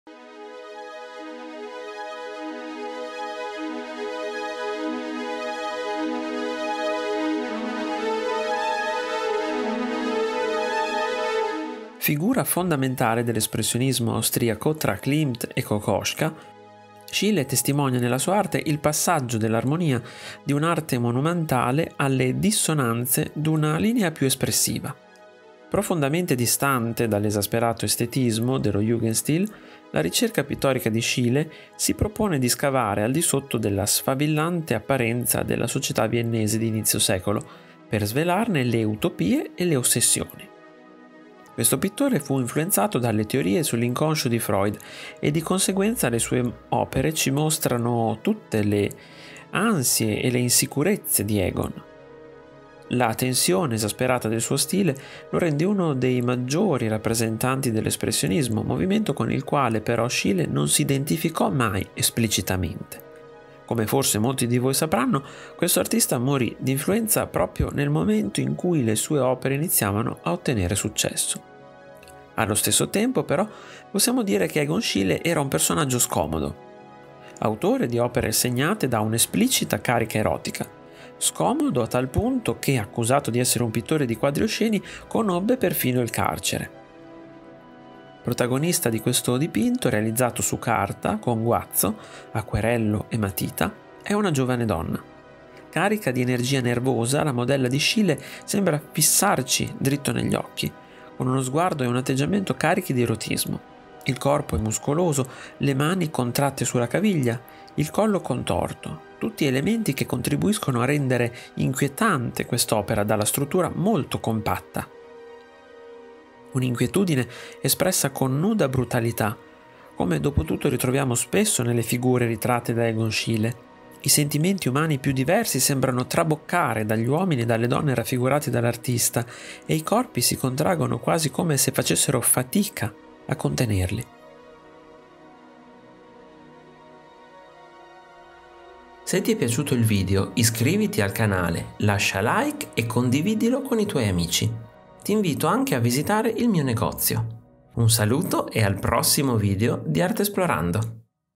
Egon Schiele | Donna seduta
L'audioquadro è un nuovo modo per conoscere i più grandi capolavori della storia dell'arte. In maniera semplice e in pochi minuti. Qui vi parlerò della Donna seduta di Egon Schiele▼▼CONTINUA SOTTO▼▼

Figura fondamentale dell’espressionismo austriaco, tra Klimt e Kokoschka, Schiele testimonia nella sua arte il passaggio dell’armonia di un’arte monumentale alle dissonanze d’una linea più espressiva. Profondamente distante dall’esasperato estetismo dello Jungendstil, la ricerca pittorica di Schiele si propone di scavare al di sotto della sfavillante apparenza della società viennese d’inizio secolo per svelarne le utopie e le ossessioni.
Questo pittore fu influenzato dalle teorie sull’inconscio di Freud e di conseguenza le sue opere ci mostrano tutte le ansie e le insicurezze di Egon. La tensione esasperata del suo stile lo rende uno dei maggiori rappresentanti dell’espressionismo, movimento con il quale però Schiele non si identificò mai esplicitamente. Come forse molti di voi sapranno questo artista morì d’influenza proprio nel momento in cui le sue opere iniziavano ad ottenere successo. Allo stesso tempo però possiamo dire che Egon Schiele era un personaggio “scomodo”, autore di opere segnate da un’esplicita carica erotica. Scomodo a tal punto che, accusato di essere un pittore di quadri osceni, conobbe perfino il carcere.

LETTURE CONSIGLIATE
➡ Egon Schiele. Diario dal carcere
➡ Schiele. Disegni erotici
➡ Ritratto d'artista